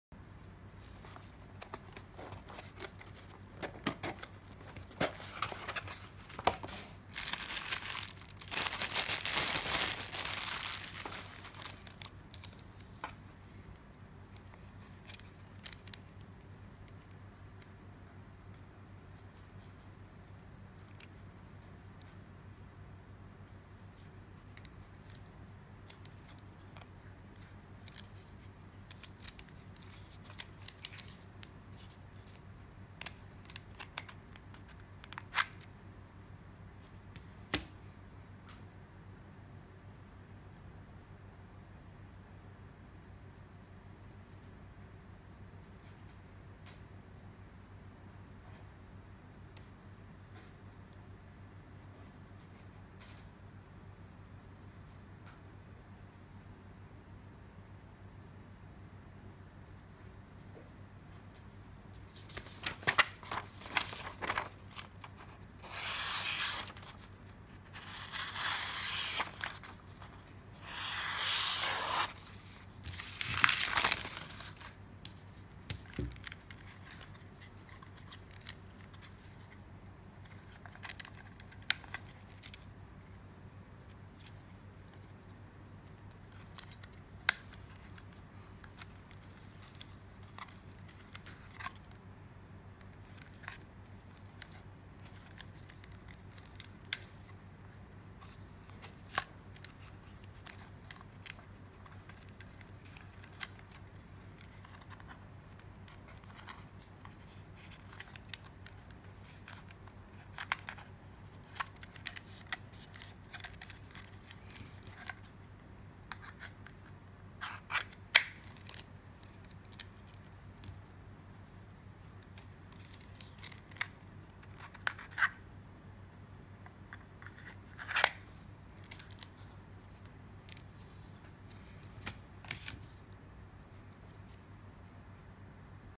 ELK RIDGE ER-200-04S FIXED BLADE KNIFE
FIXED BLADE KNIFE
5.57" OVERALL
3.5" 3CR13 STEEL BLADE
2.25" G10 HANDLE
INCLUDES KYDEX SHEATH WITH BELT LOOP